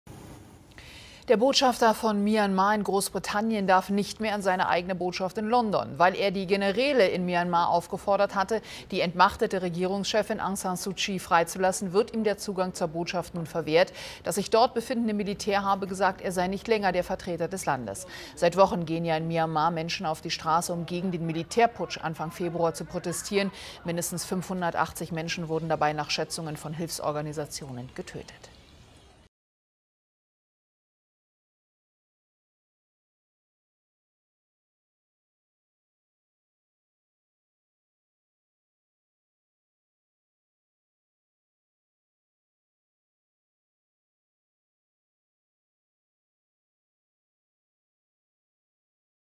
KEIN ZUTRITT: Botschafter von Myanmar wird aus eigener Vertretung in London ausgesperrt
Myanmars Botschafter in Großbritannien, ein erklärter Gegner der Militärjunta in seinem Land, ist von seiner eigenen Botschaft in London ausgesperrt worden. Kyaw Zwar Minn sei zu dem Gebäude im Zentrum der britischen Hauptstadt der Zutritt verwehrt worden, berichteten mehrere Nachrichtenagenturen am Donnerstagmorgen. Der Militärattaché habe ihn aufgefordert, das Gebäude zu verlassen, er sei nicht länger der Vertreter des Landes, zitierte die BBC den Botschafter. Die britische Regierung verurteilte den Vorfall scharf.

Dem „Telegraph“ sagte Kyaw Zwar Minn demzufolge: „Sie sagten, sie hätten eine Anweisung aus der Hauptstadt erhalten, deshalb würden sie mich nicht reinlassen.“ „Dies ist Großbritannien, wir sind nicht in Myanmar.“ Die britische Regierung werde das nicht zulassen. Kyaw Zwar Minn forderte die Regierung auf, einzuschreiten. Das sei ein „Putsch“ des „myanmarischen Militärs“. Vor der Botschaft versammelten sich am Mittwoch Demonstranten, nachdem Medien berichtet hatten, dass der Botschafter am Betreten des Gebäudes gehindert worden war.